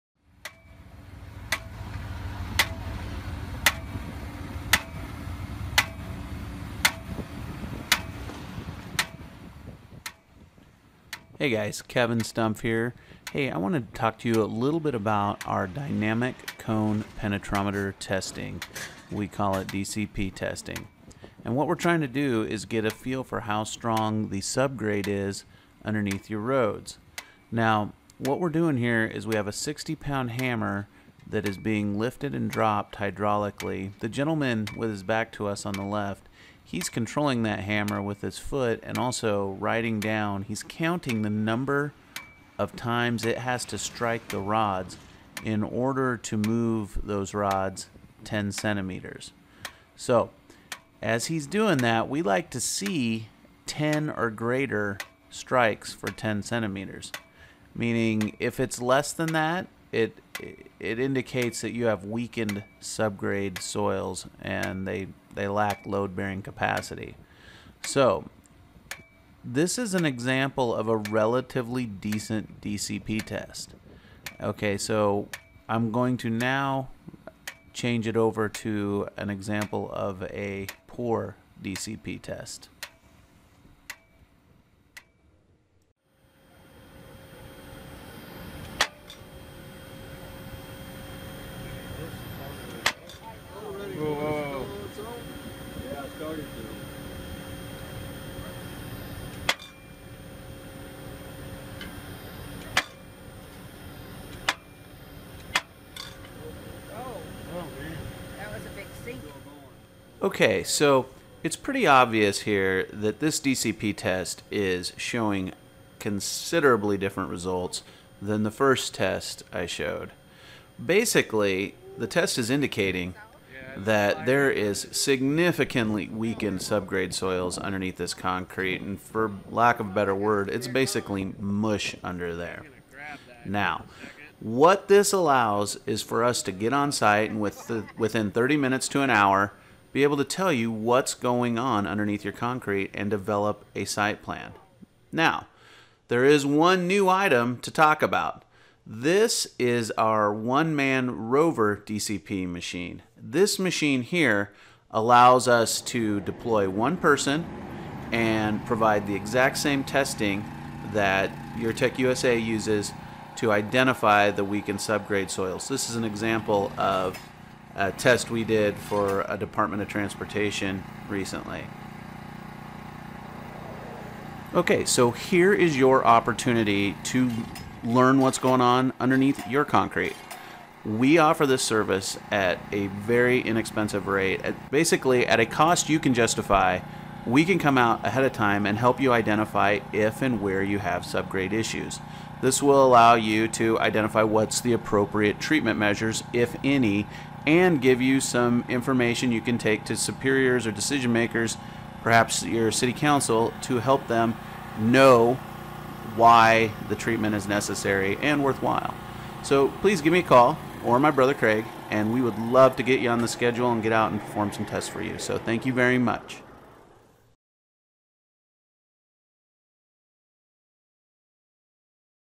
Onsite Discovery - Dynamic Cone Penetrometer Testing
This short video gives an overview of why DCP Testing is beneficial to Street and Highway Engineers.